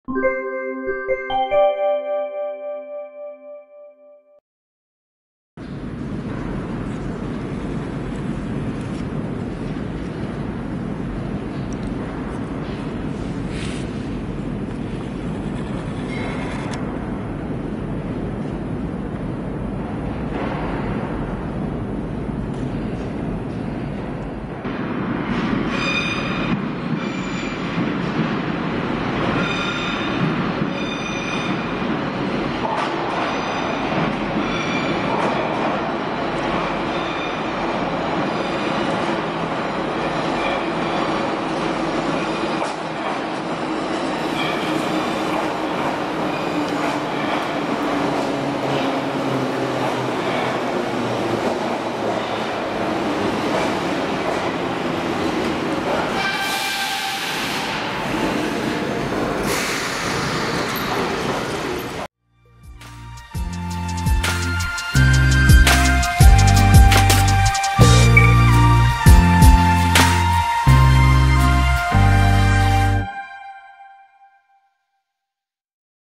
"Из тупика!" Метропоезд Ем-501/Ема-502 (81-501/502) №121 на ст.м.Площадь Ленина, фев.2023г.
Метропоезд Ем-501/Ема-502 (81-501/502) (Приписан к электродепо ТЧ-1 Автово Петербургского метрополитена) маршрут №121 -
"Площадь Ленина - Проспект Ветеранов".

Прибывает на станцию метрополитена Площадь Ленина из тупика, Кировско-Выборгская линия (КВЛ, М1) Петербургского метрополитена.
4.II.2023